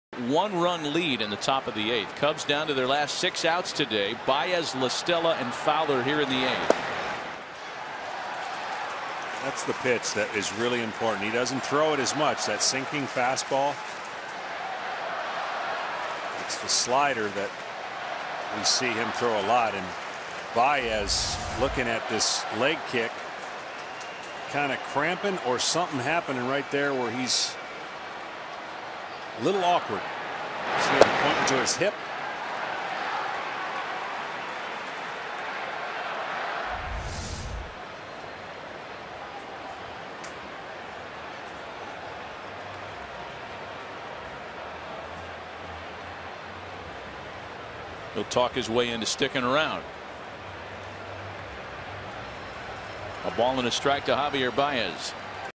CHC@HOU: Baez gets visit from the trainer, stays in
Javier Baez gets a visit from the Cubs trainer while batting in the 8th inning